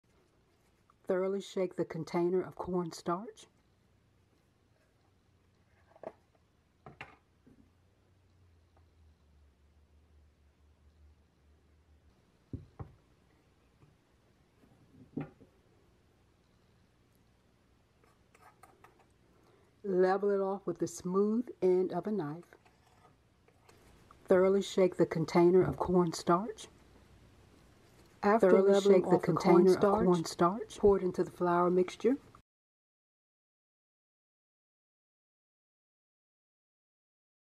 How To Measure Corn Starch
This is how I measured two tablespoons of corn starch (also spelled cornstarch) for my Homemade Flour recipe:

1.  Shake up the container of corn starch.
2.  Scoop out a heaping tablespoon full.
3.  Level off top with knife's smooth end.